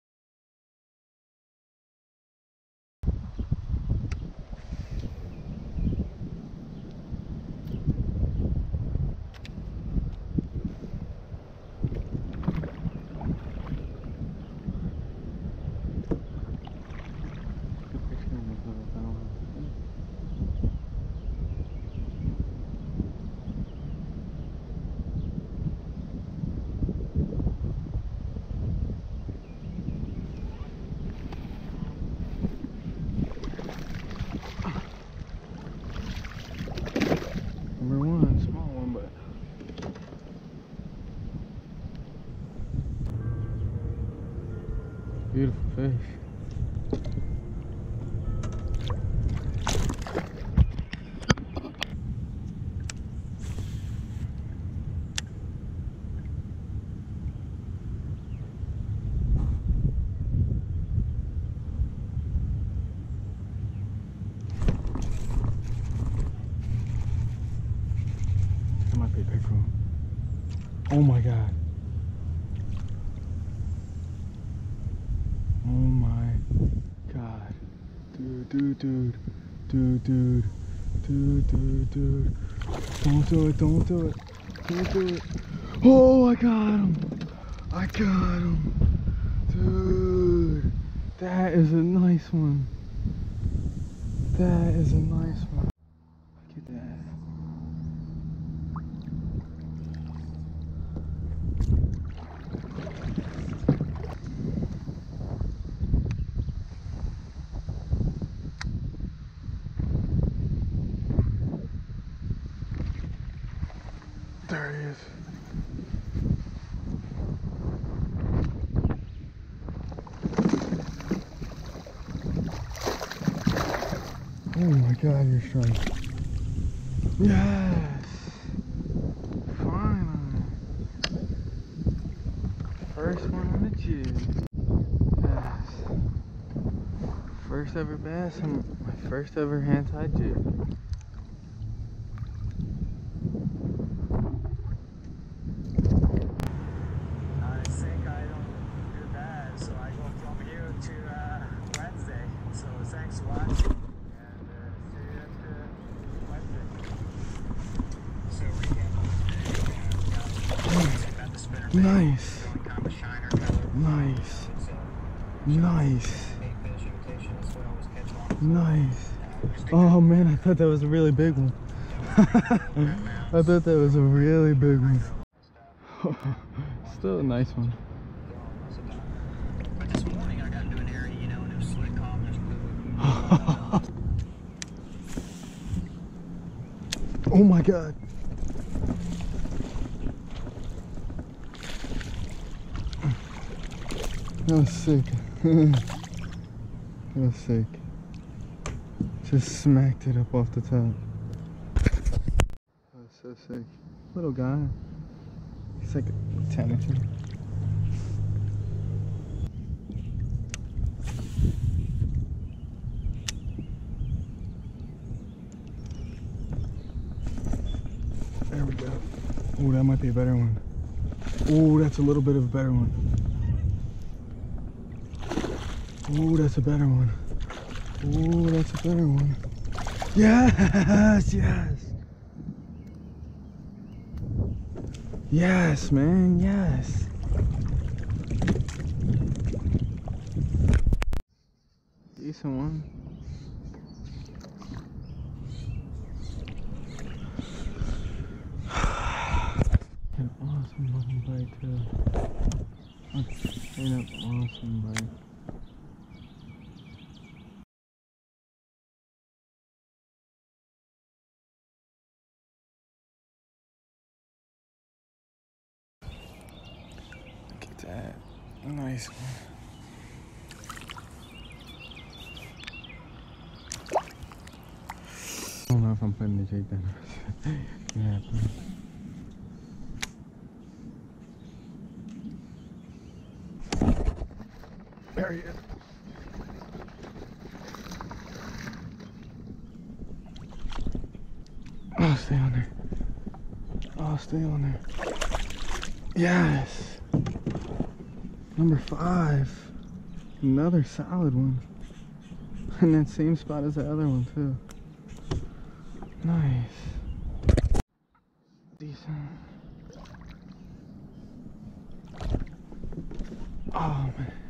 Bass Fishing at Vincentown Millpond
Got lucky and actually caught a couple. I'm kinda mad cause I lost that jig I tied to a fucking snapping turtle of all things.